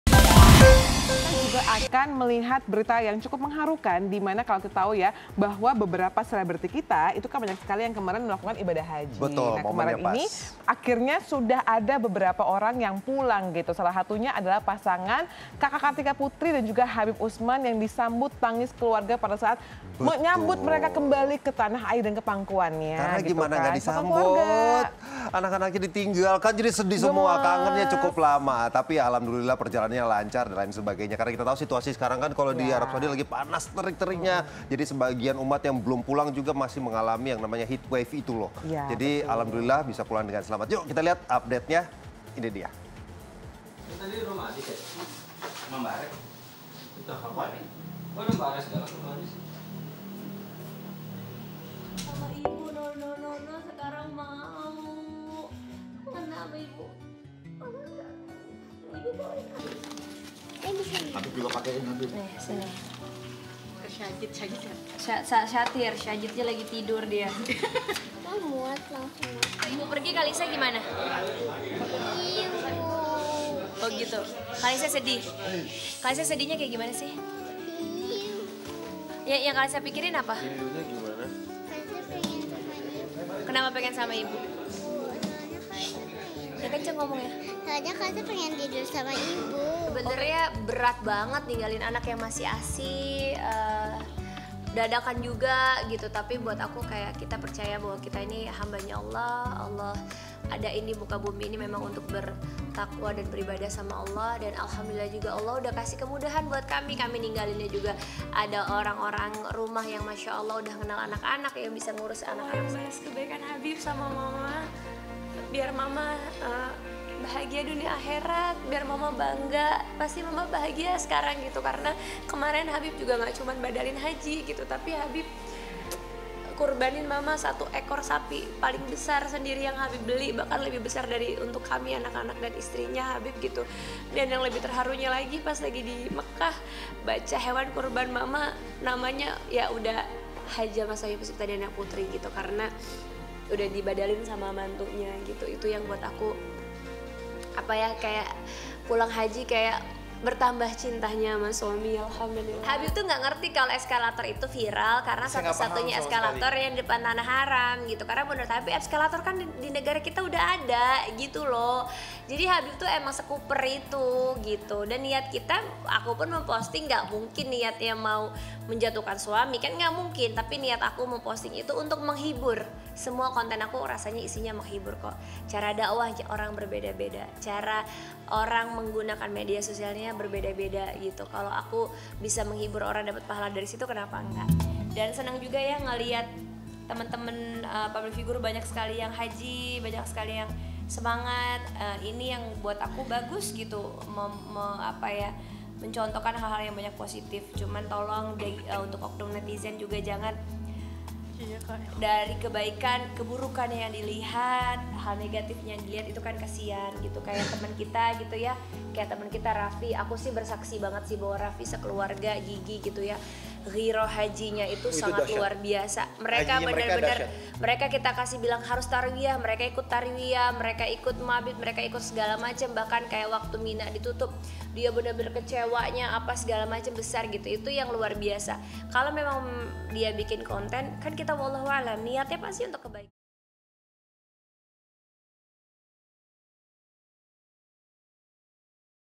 Kartika Putri Pulang dari Makkah, Langsung Disambut Tangis Anak-anak
Kartika Putri dan Habib Usman Bin Yahya baru saja selesai beribadah haji. Sudah pasti kedatangan mereka langsung disambut oleh anak-anaknya. Kartika juga menceritakan bahwa dirinya jalani bidal Haji untuk almarhum ibunda.